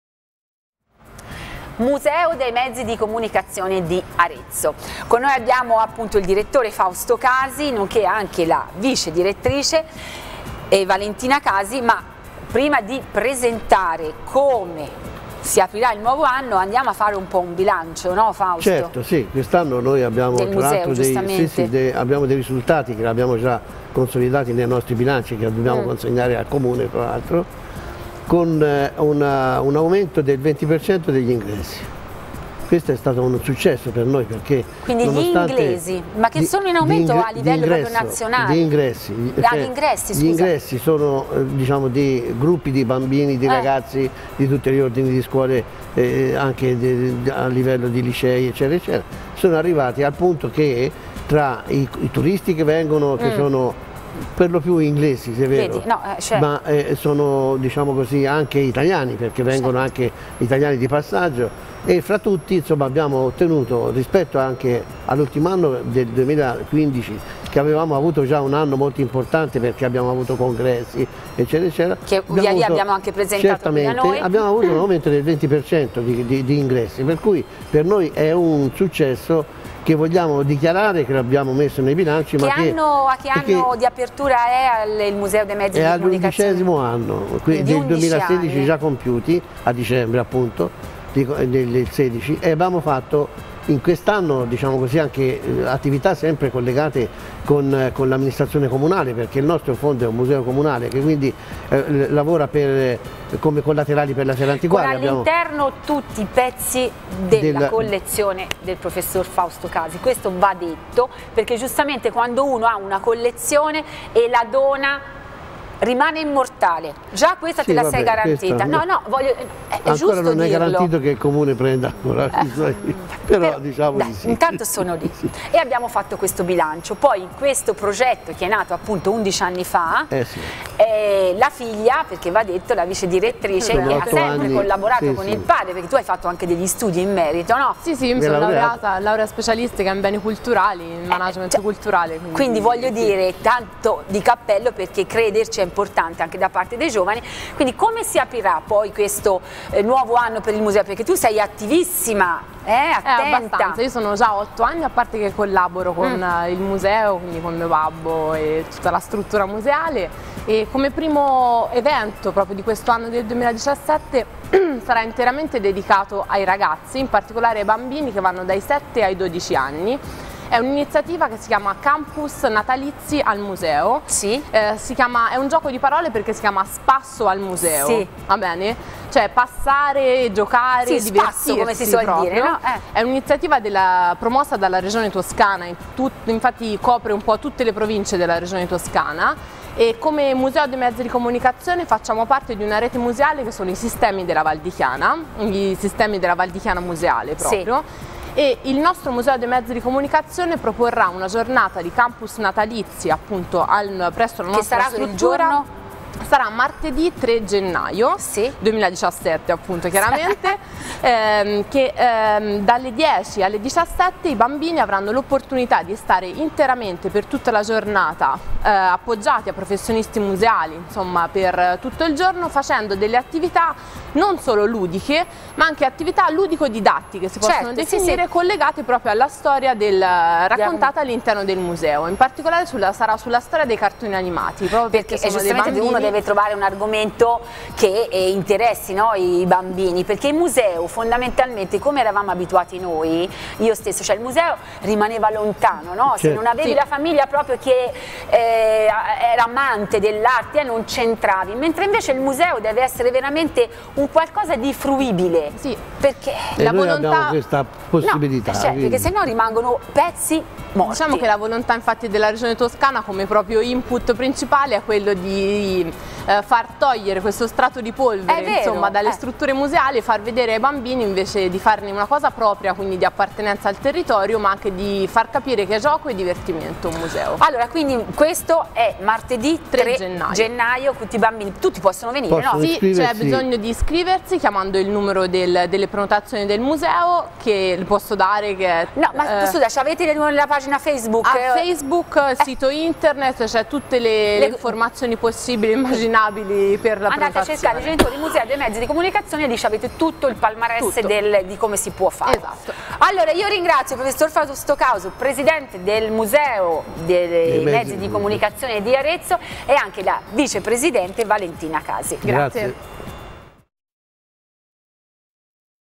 MMC AREZZO @ FOCUS Teletruria - Campus Natalizi al Museo e bilancio attività 2016
Il Direttore del Museo dei Mezzi di Comunicazione, Prof. Fausto Casi, e la Vicedirettrice, Dott.ssa Valentina Casi, ospiti su FOCUS-Teletruria per parlare del bilancio dei 32 eventi organizzati dalla struttura museale nell'anno 2016 e per presentare la prima attività dell'anno 2017: CAMPUS NATALIZI AL MUSEO - un'iniziativa della Regione Toscana ed Edumusei portata per la prima volta nella provincia di Arezzo dal Sistema Museale - Valdichiana Musei